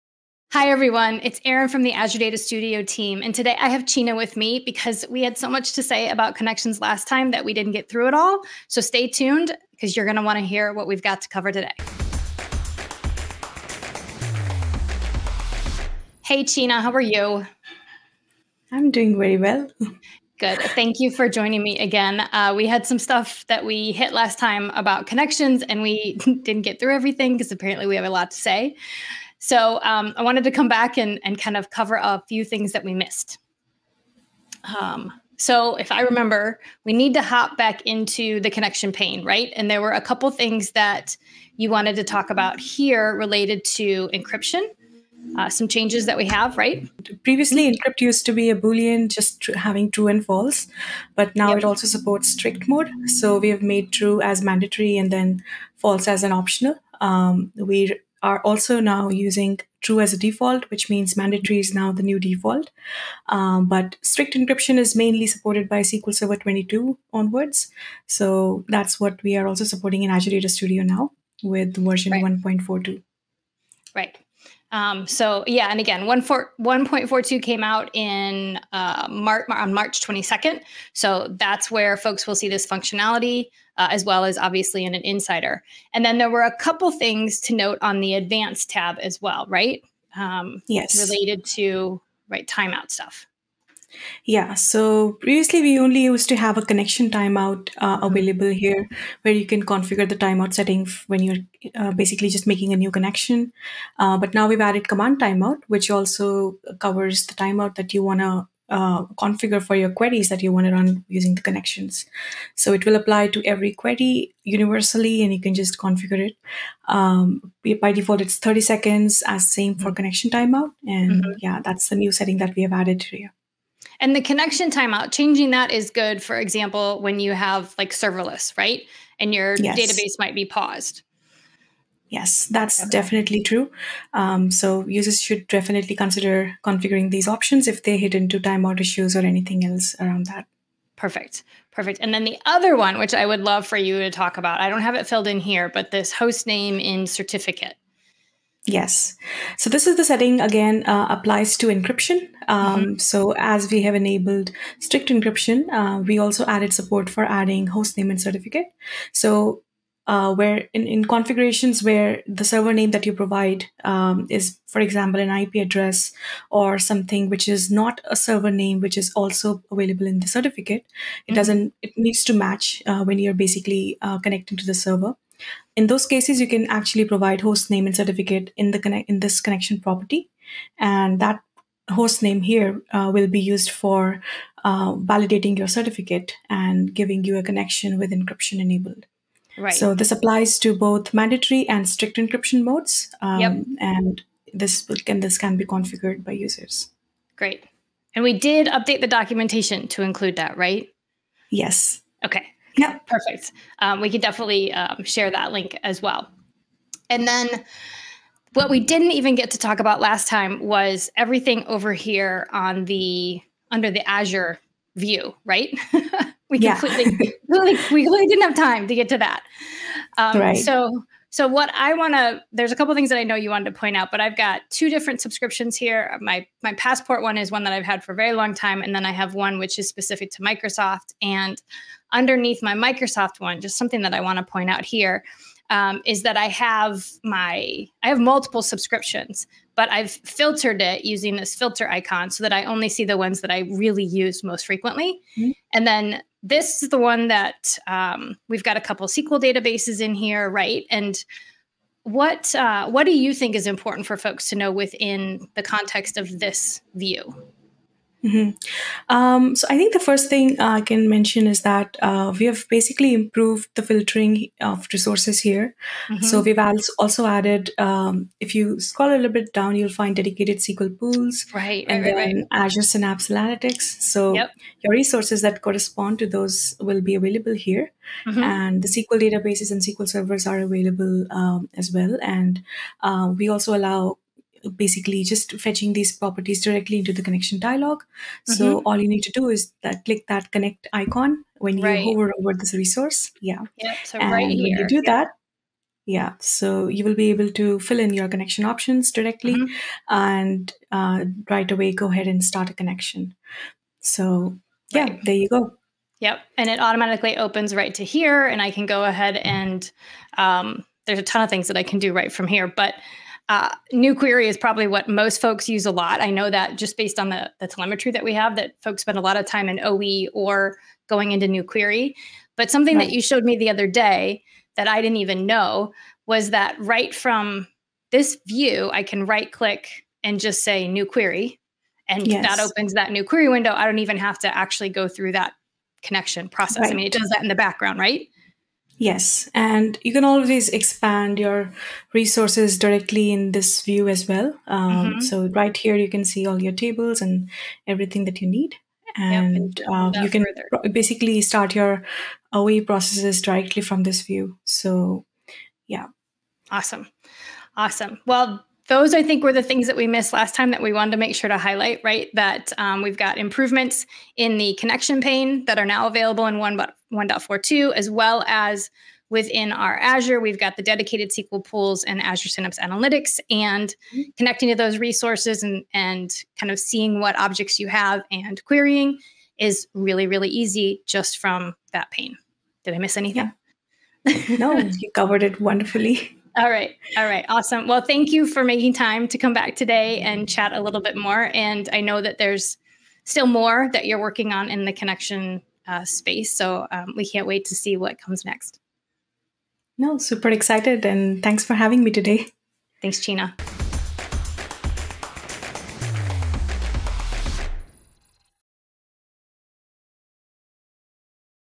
Managing Connections in Azure Data Studio, Part 2
In this video, Erin Stellato (PM for Azure Data Studio) and Cheena Malhotra (Senior Software Engineer) continue their conversation about Connectivity in Azure Data Studio, highlighting additional settings and walking through the Azure view which allows users to explore resources within their Azure subscription.

00:00:00 Introduction
00:00:43 New Connection Features
00:03:30 Azure View